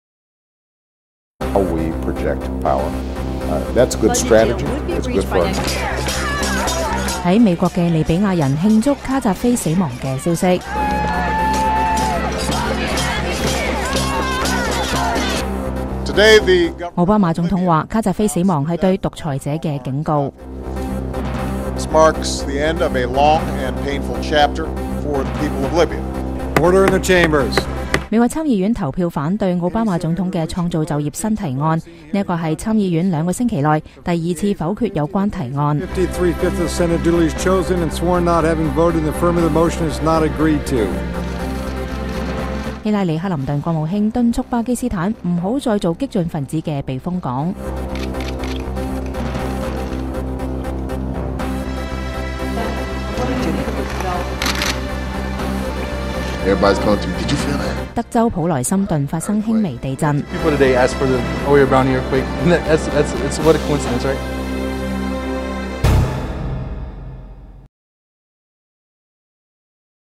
VOA美國60秒(粵語): 2011年10月21日
1.在美國的利比亞人慶祝卡扎菲死亡的消息
2. 奧巴馬總統說卡扎菲死亡是對獨裁者的警告
3.美國參議院投票反對創造就業新提案
4.克林頓敦促巴國別再做激進分子避風港
5.德州普萊森頓發生輕微地震